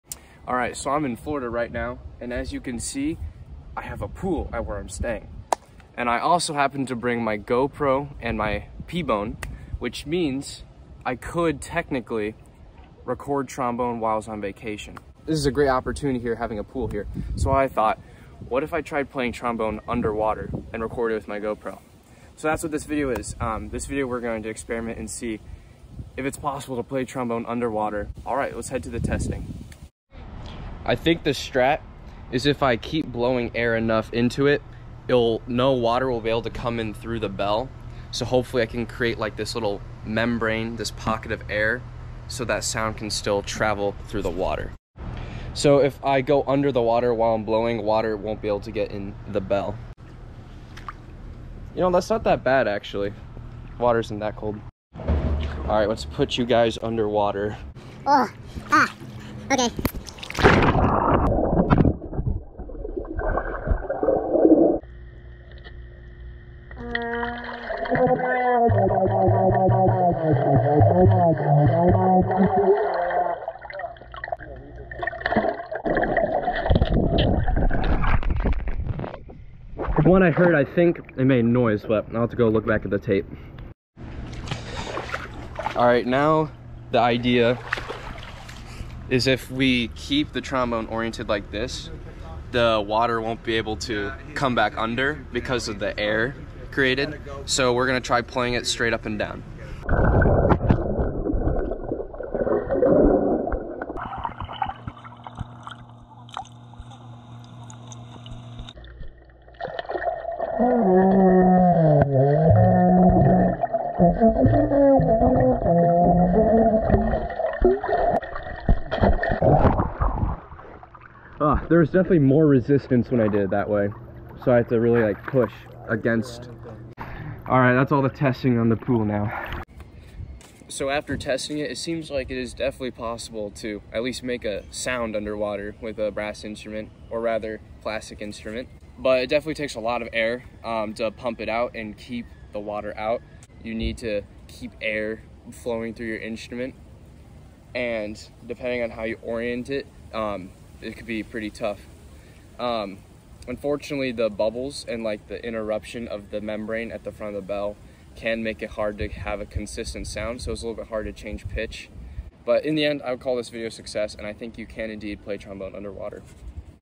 Can you play the trombone underwater?
Taking advantage of the pBone I brought on our family vacation to Marco Island, FL, I try and find out how easy (or not) it is to play trombone underwater.